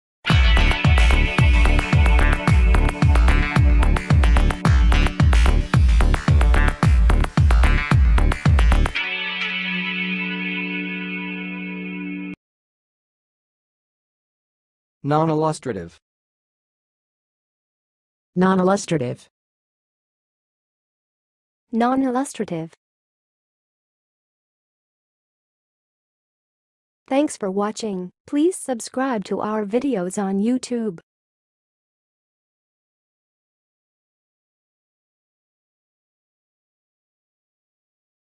HOW TO PRONOUNCE NONILLUSTRATIVE?
NONILLUSTRATIVE - The definitive pronunciation guide for Nonillustrative in 2023

How should one pronounce the word nonillustrative correctly?
What is the stress pattern in the pronunciation of nonillustrative?
What is an alternative pronunciation of nonillustrative?
How can one improve their pronunciation of nonillustrative?
Are there any common mispronunciations of nonillustrative?
NONILLUSTRATIVE Meaning And Definition:
Nonillustrative refers to something that does not serve as an example or provide visual representation.
NONILLUSTRATIVE Pronunciation:
To pronounce nonillustrative, break it down into syllables: NON-ILL-US-TRAY-TIV, with the stress on the second syllable.
Alternatively, you can pronounce it as NON-ILL-US-TRUH-TIV, with the stress on the third syllable.
Use NONILLUSTRATIVE In Your Speech:
1. The professor used a nonillustrative example during the lecture, which made it difficult for the students to understand the concept
2. The book's illustrations were so nonillustrative that it was hard to visualize the story
3. The chart presented in the report was nonillustrative and did not effectively demonstrate the data
Please answer in comments for the most popular NONILLUSTRATIVE questions:
1. What are some common examples of nonillustrative concepts in philosophy?
2. How can nonillustrative visuals impact the effectiveness of educational materials?
3. When should one avoid using nonillustrative examples in presentations?
4. What are the characteristics of a nonillustrative diagram?
5. How can nonillustrative representations hinder communication?